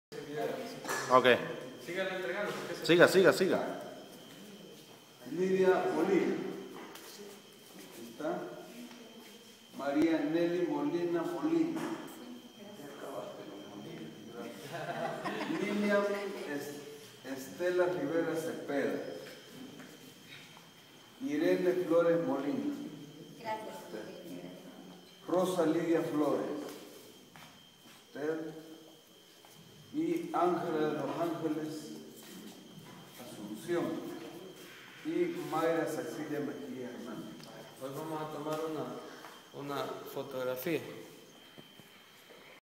ARENA. Diputado Aristides Corpeño, Diputado por La Paz , hace entrega de documentos de propiedad
Como respaldo del Diputado Arístides Corpeño, en coordinación con el equipo de Arena 3.0, este día se entregaron documentos que acreditan la legalidad de posesiones materiales de habitantes del Cantón Concepción Lourdes en el Municipio de San Emigdio, Departamento de La Paz, en su cuarta etapa. Los habitantes de dicha comunidad recibieron con mucha alegría la colaboración prestada y agradecieron al Alcalde y al Diputado por su esfuerzo realizado.